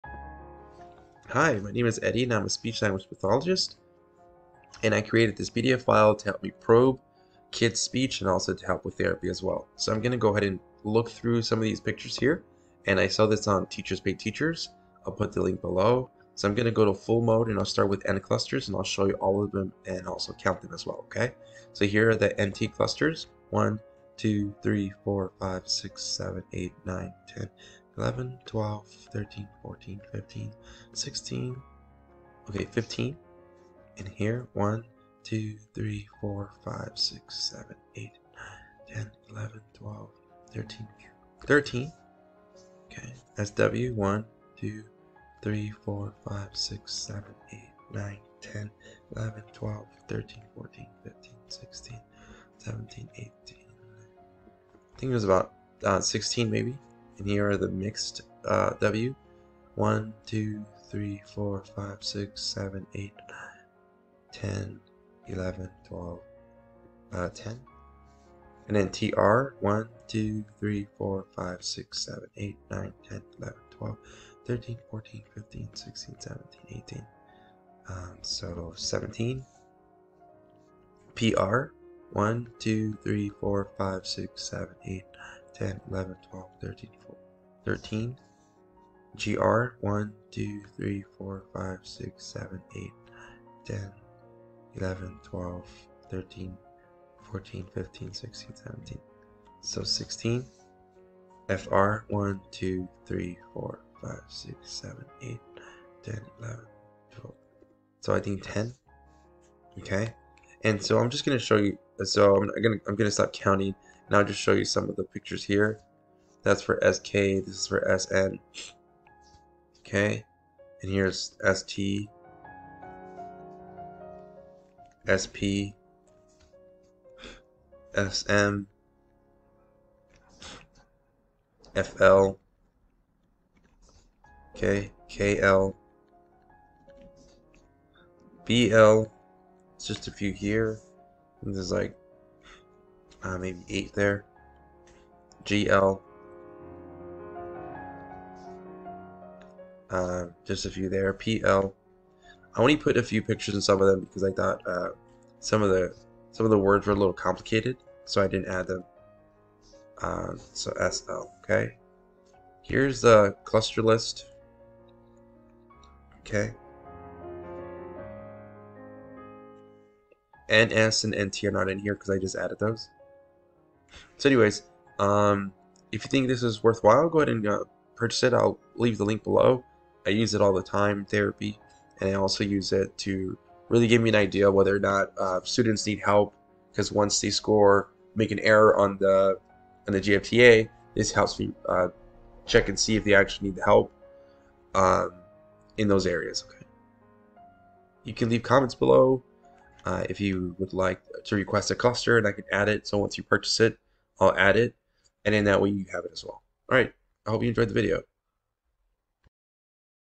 Articulation Clusters / Cluster Reduction
I use this at work to probe and use as therapy material. It is an easy way to probe and get lots of trials.